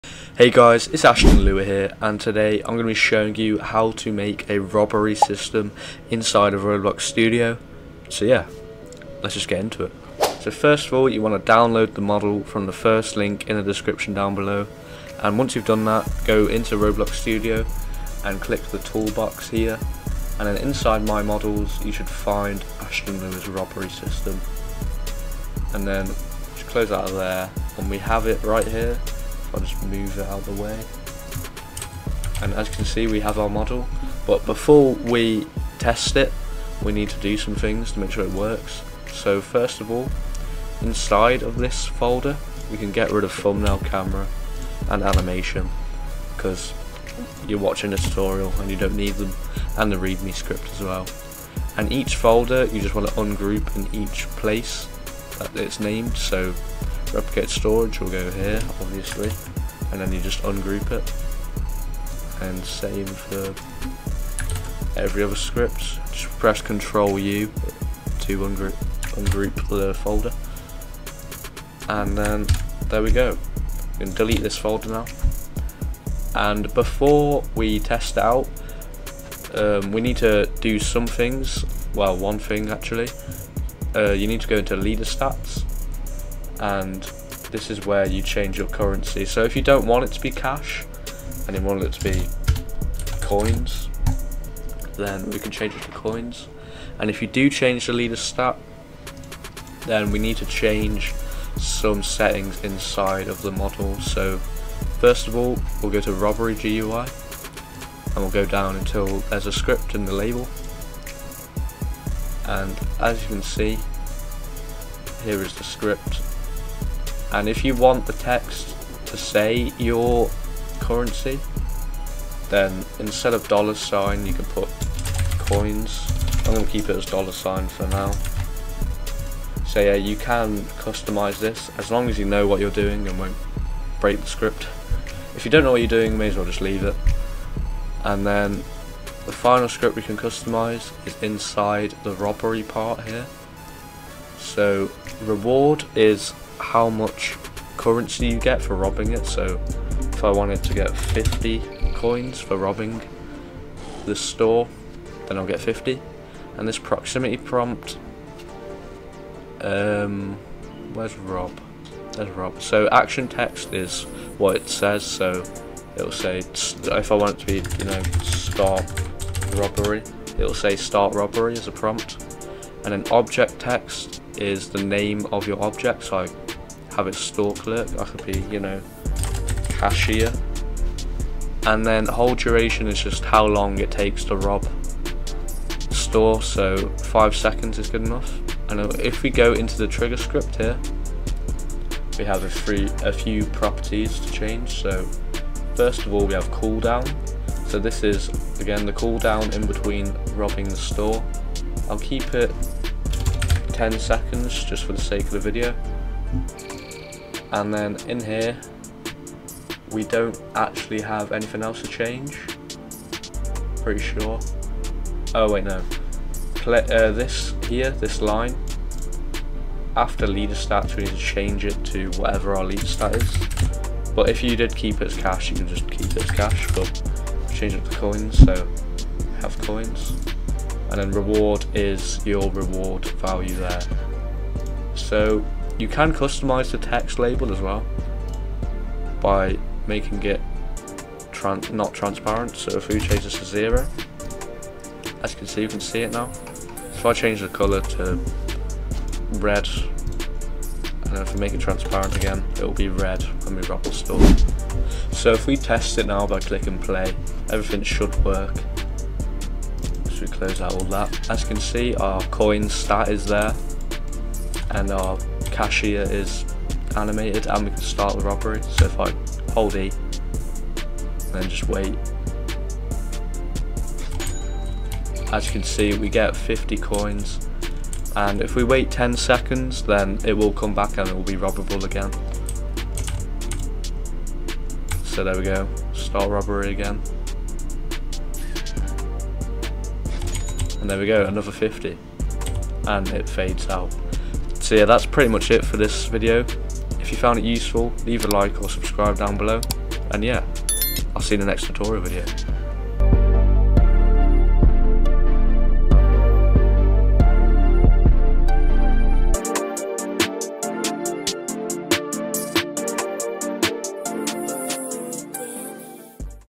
How To Make A ROBBERY SYSTEM - Roblox Studio
In this video, I'll show how to download and setup my Roblox robbery system!  😊